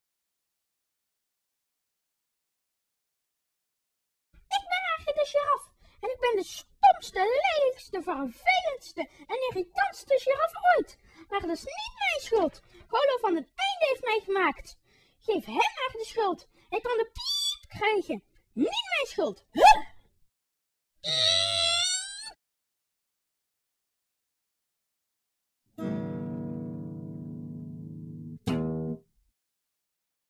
Arie de Giraffe
Tweede oefening bij Demian: morphing (in klei-animatie). Arie de Giraffe wordt, van louter woede, een... gitaar! - Drie uur werk voor 30 seconden film.